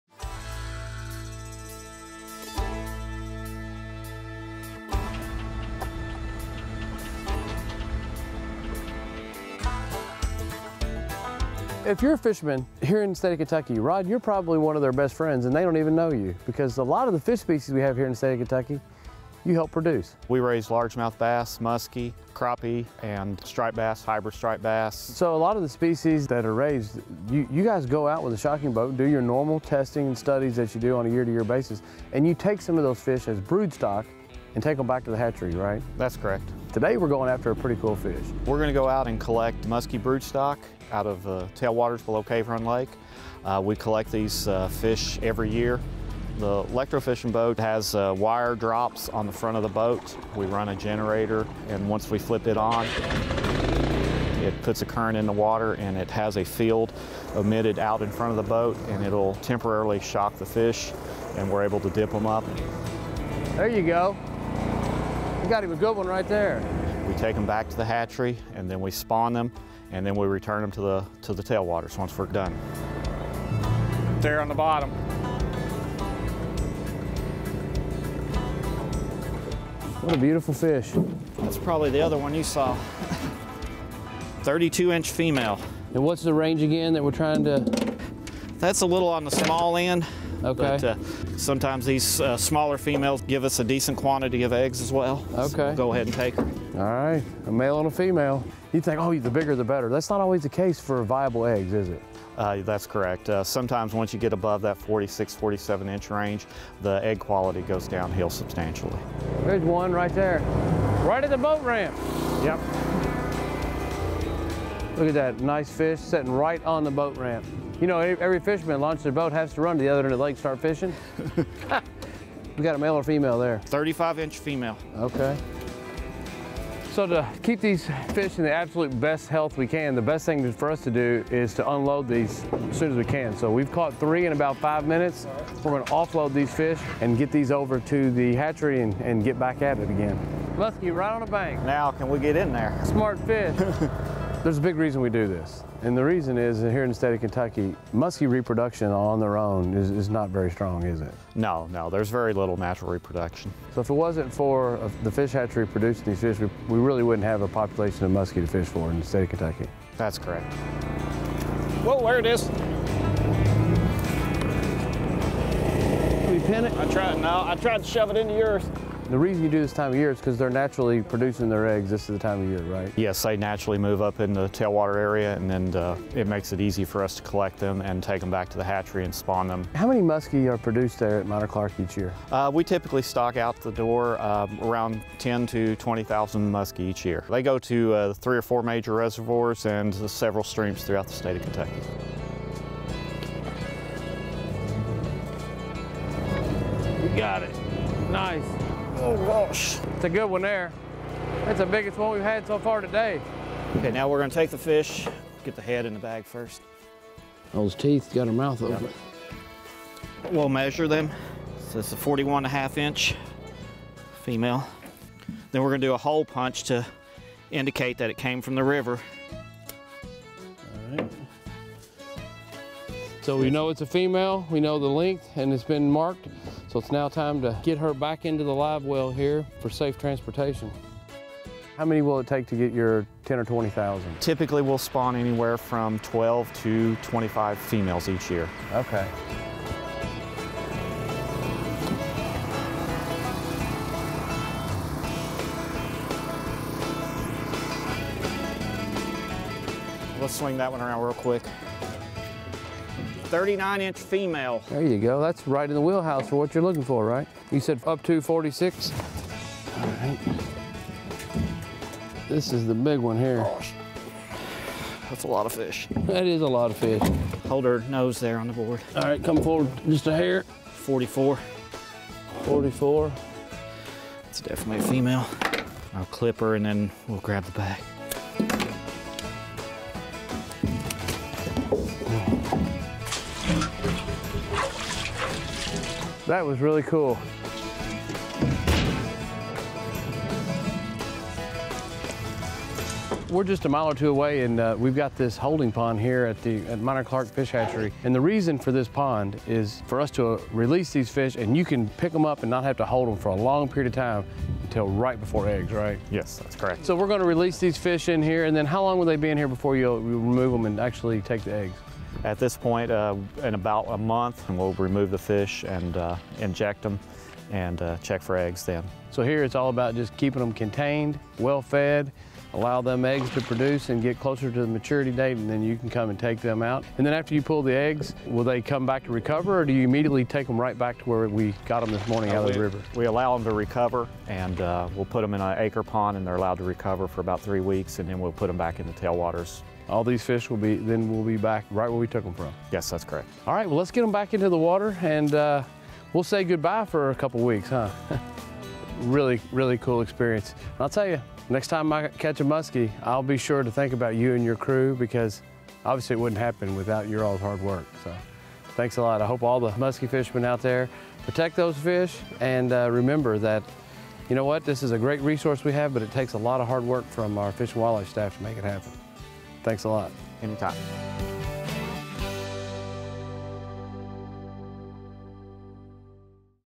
Electrofishing Musky for Broodstock - Where Kentucky's Musky Come From
Today we're on the Licking River with KDFWR fisheries biologist, Rod Middleton and we're collecting musky for the broodstock program. Musky do not naturally reproduce in the state of Kentucky, so practically all of the states fish are stocked... This is where they come from!